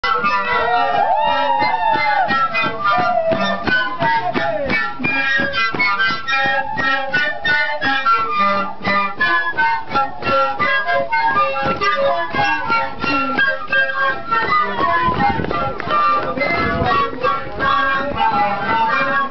Tinku Dancing 2008
Me as a Tinku dancer during Peace Corps service in Bolivia. I'm in there, just look closely for the only gringo.